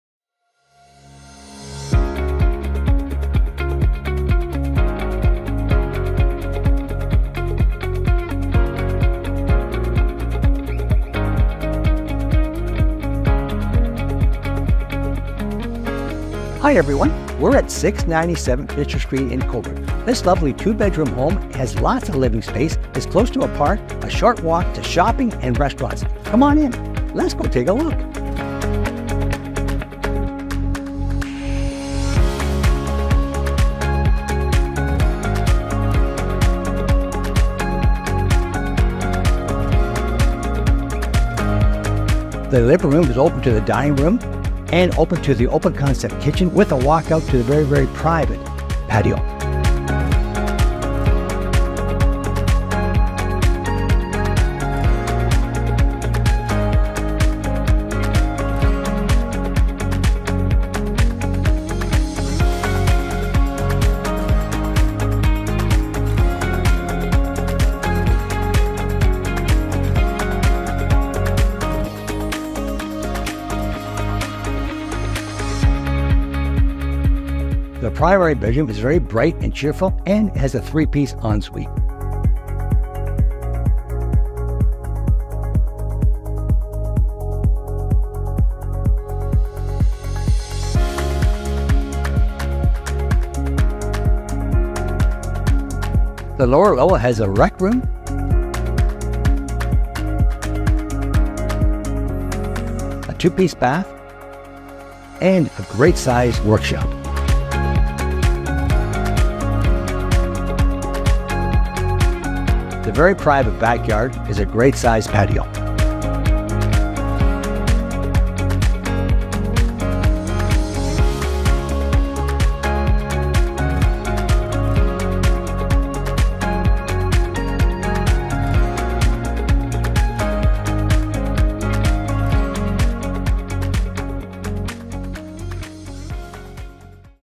697 Fisher St, Cobourg | Open House Video Tour - Unbranded
Nestled in a prime location close to a park, shopping, and dining, this charming 2-bedroom, 3-bathroom residence is the epitome of comfort and convenience. Step inside to discover a home that has been meticulously upgraded and enhanced to offer you the ultimate living experience. The spacious living room welcomes you with abundant natural light, creating an inviting atmosphere for relaxation and social gatherings. Entertain guests with ease in the formal dining room, where every meal becomes a memorable occasion. The gourmet kitchen is a chef's delight, boasting ample cabinet space, bright interiors, and a convenient walkout to your own private backyard retreat. Retreat to the primary bedroom, a haven of tranquility featuring a bright and cheerful ambiance, along with a custom3-piece en suite for your comfort and convenience. Downstairs, the lower level beckons with a cozy rec room, a convenient 2-piece bath, and a generously sized workshop area offering plenty of storage space for your hobbies and belongings. Step outside to your own private oasis, where a custom patio awaits, perfect for al fresco dining, entertaining, or simply unwinding and enjoying the evening sunsets. Don't miss your chance to make this exquisite home yours. Schedule a viewing today and experience the perfect blend of style, comfort, and convenience in one delightful package.

Mls X8368524